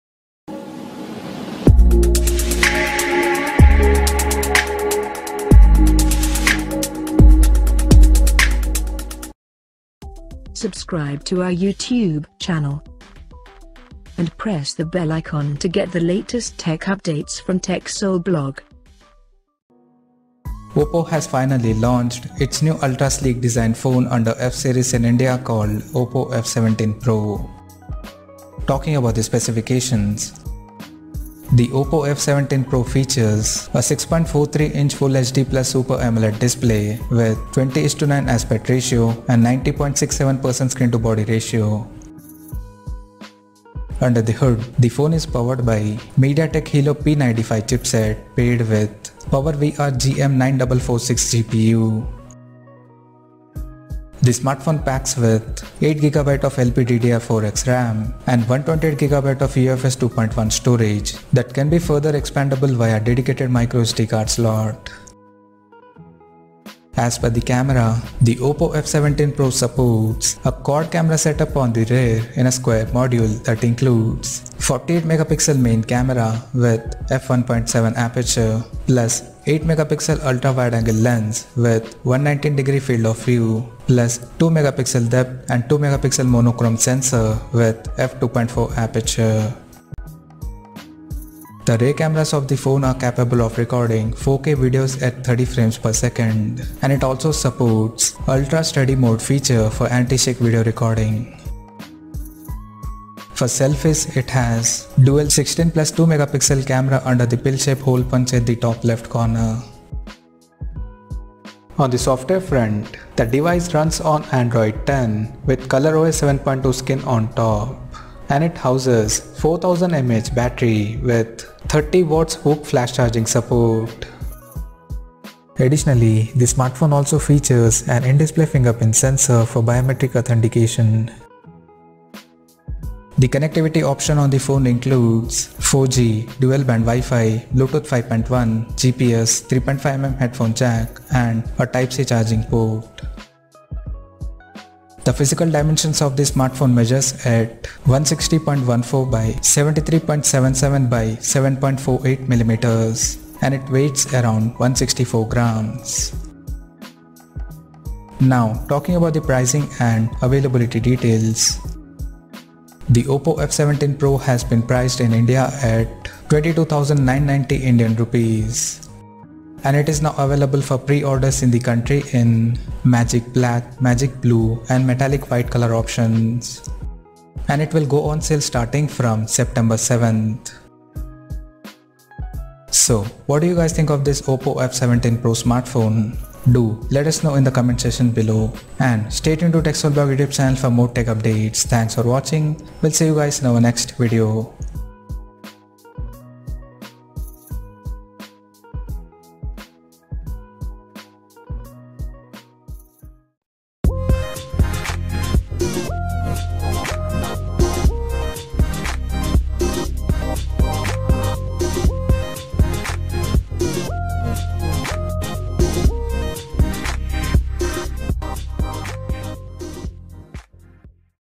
Oppo F17 Pro Price, Official Look, Design, Camera, Specifications, 8GB RAM, Features & Sale Details
Oppo F17 Pro Price, Official Look, Design, Camera, Specifications, 8GB RAM, Features and Sale Details

Oppo F17 Pro and Oppo F17 have been launched in India with quad rear cameras. The new phones also come with Oppo's proprietary 30W fast charging technology and feature three distinct colour options. On the part of differences, the Oppo F17 Pro offers dual selfie cameras, while the Oppo F17 has a single camera sensor at the front for capturing selfies. The Oppo F17 Pro in the lineup also comes with a Super AMOLED display. The Oppo F17 series phones come in a metal finish design, to attract young customers, according to the brand. The Oppo F17 Pro runs on Android 10 with ColorOS 7.2 on top and features a 6.43-inch full-HD+ (1,080x2,400 pixels) Super AMOLED display with a 20:9 aspect ratio and a 90.7 percent screen-to-body ratio. There is also a standard, 60Hz refresh rate on the display that has a hole-punch design. Source

What do you think of this Oppo F17 PRO?